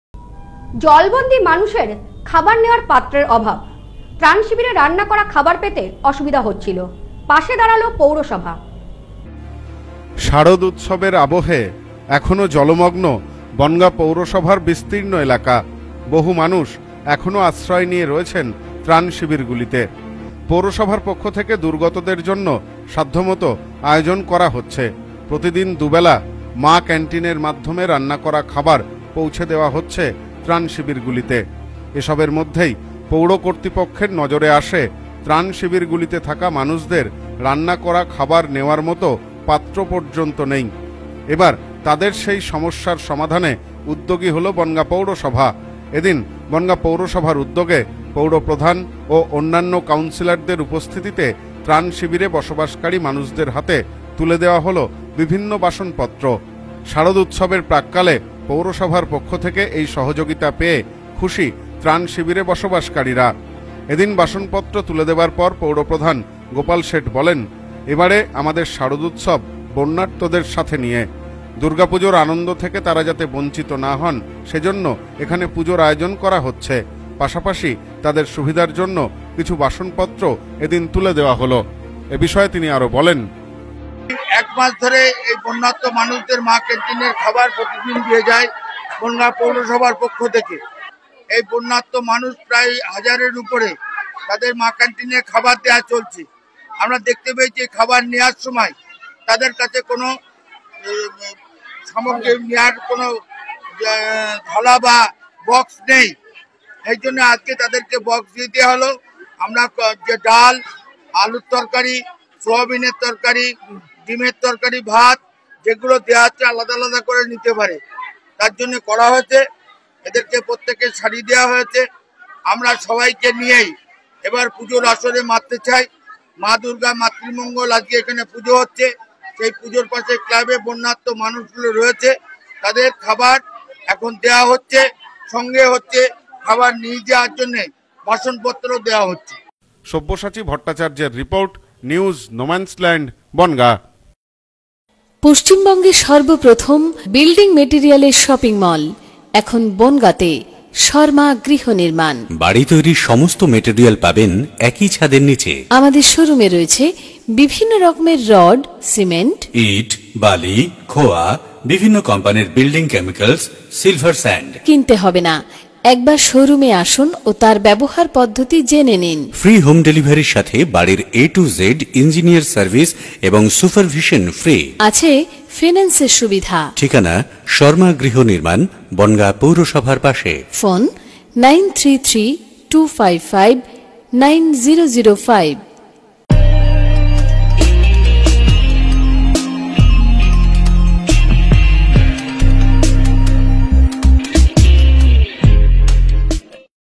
জলবন্দি মানুষের খাবার নেওয়ার পাত্রের অভাব। পাশে দাঁড়ালো পৌরসভা।
জলবন্দি মানুষের খাবার নেওয়ার পাত্রের অভাব। ত্রান শিবিরে রান্না করা খাবার নিতেও অসুবিধায় পড়তে হচ্ছিল তাদের। পাশে দাঁড়ালো পৌরসভা।